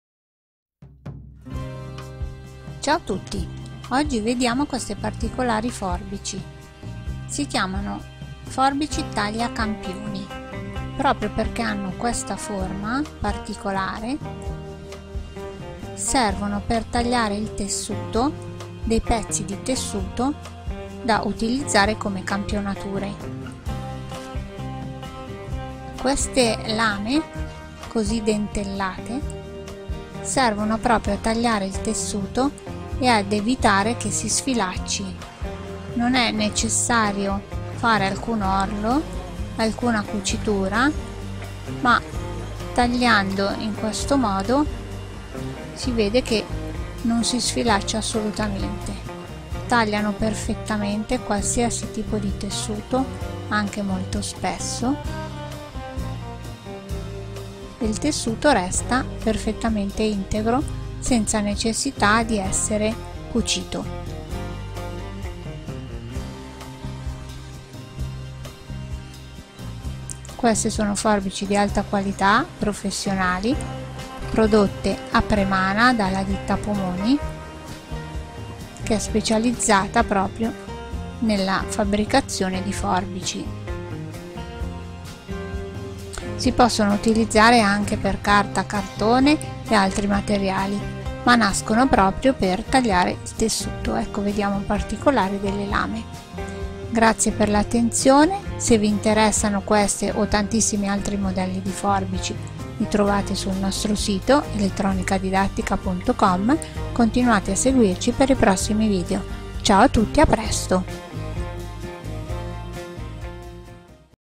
Forbici tagliacampioni. Ecco delle forbici per sartoria e settore tessile: le forbici taglia zig zag
In sartoria e nel settore tessile le forbici tagliacampioni a zig zag sono conosciute ed usate. Proponiamo delle forbici prodotte in Italia da abili artigiani, si tratta delle forbici usate nel settore tessile e chiamate tagliacampioni.